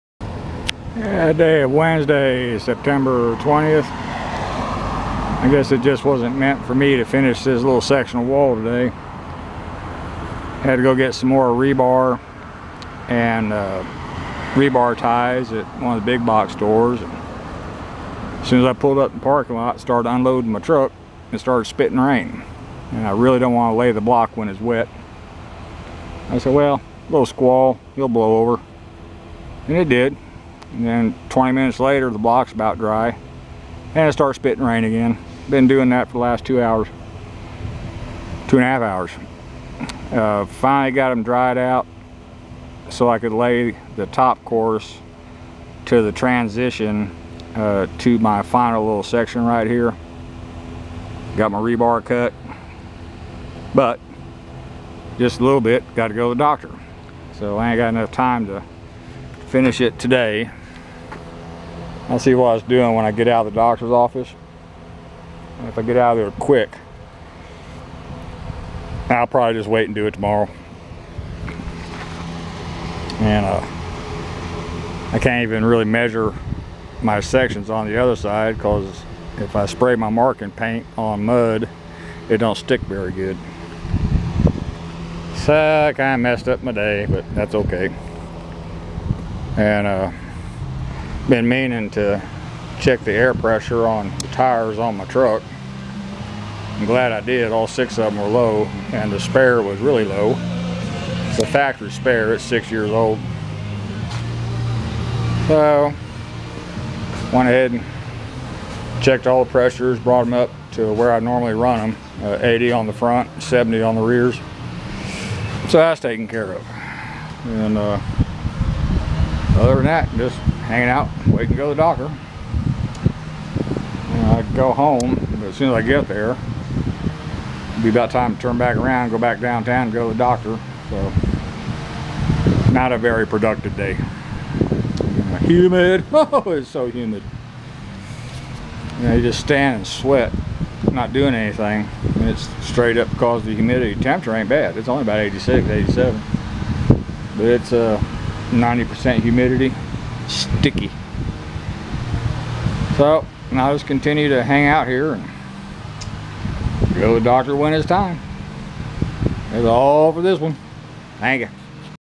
20 Sept Little building rain out again.
got very little done but did check tire pressures. I run 80 on the front and 70 on the rear duals.